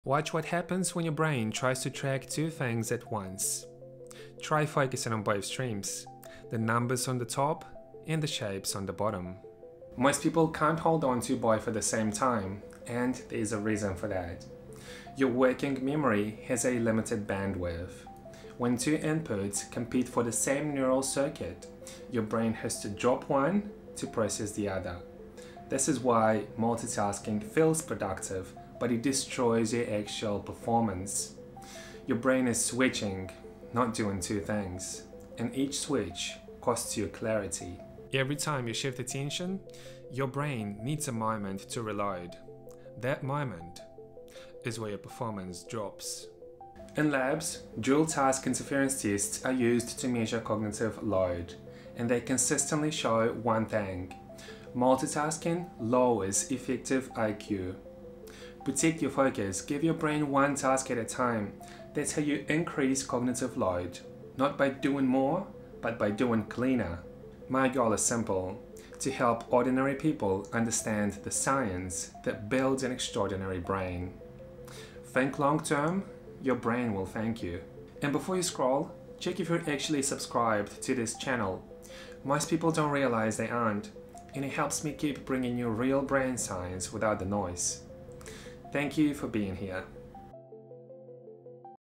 Dual-Task Challenge: Multitasking Destroys Performance
Multitasking feels productive, but your brain tells a different story.
This quick test shows what actually happens when you try to track two things at once, and why your performance quietly collapses.